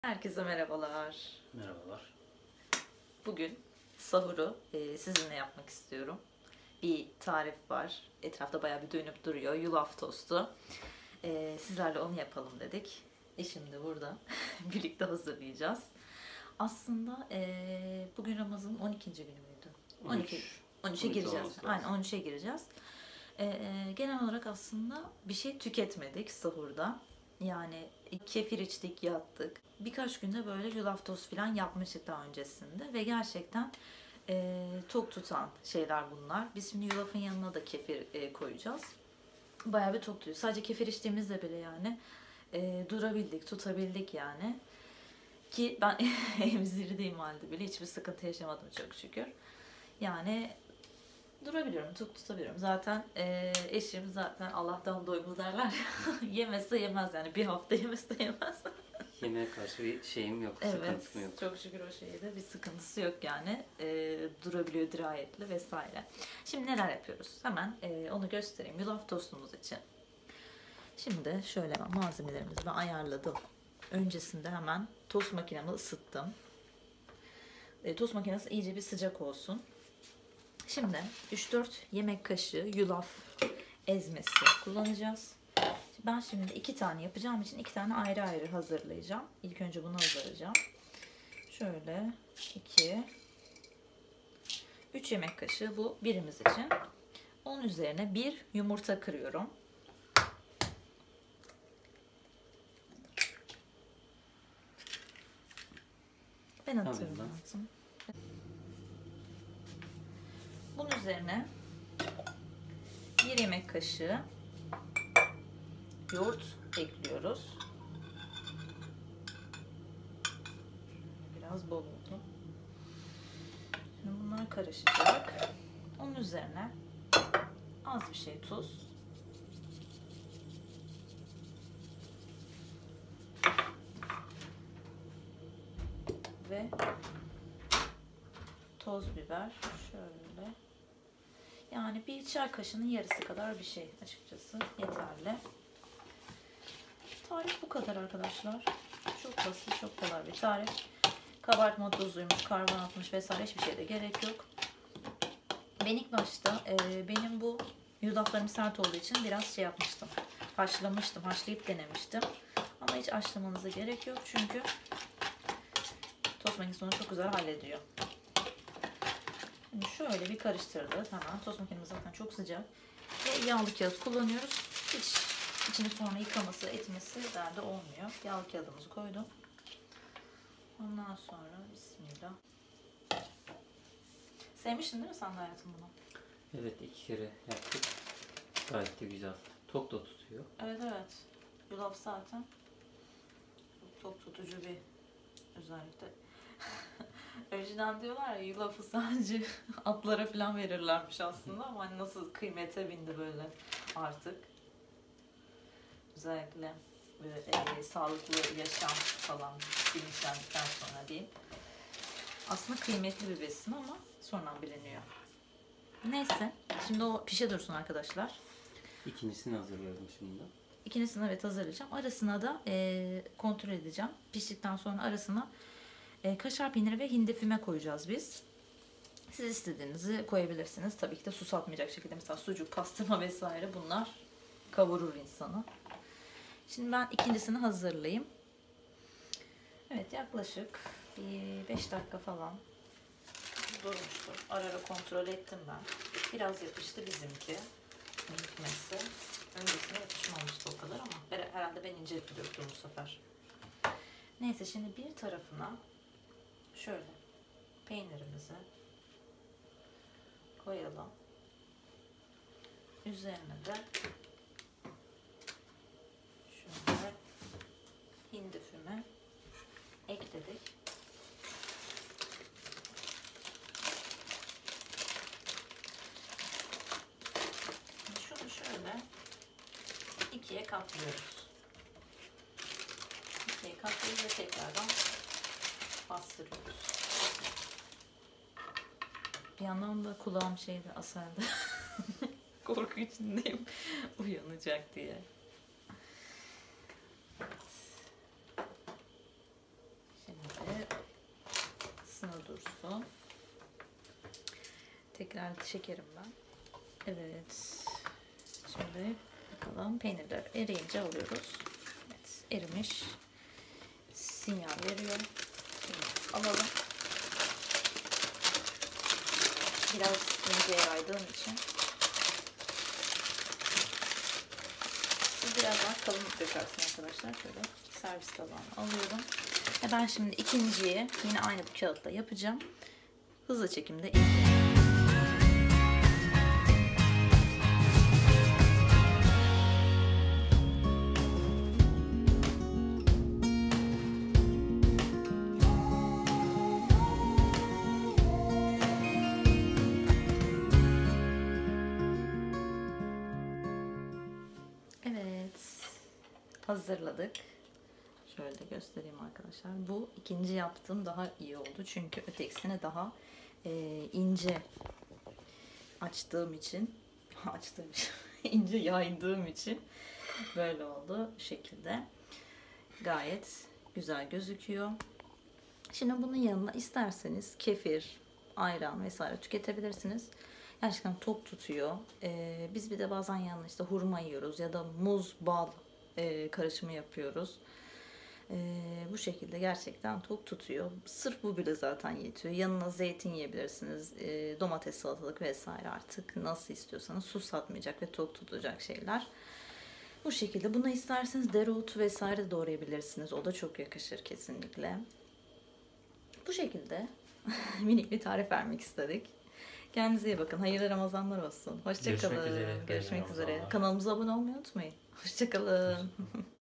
YULAF TOST-Tok Tutan Sahur Tarifi-Sadece 3 Malzeme
Merhabalar, bu videomda son dönemde oldukça trend olan bir yiyeceği yaptım. Yulaf tost. Kendimden de bir şey katarak yaptığım bu tostu mutlaka deneyin. Hem çok kolay hem az malzemeli hem de tok tutuyor. Keyifli seyirler.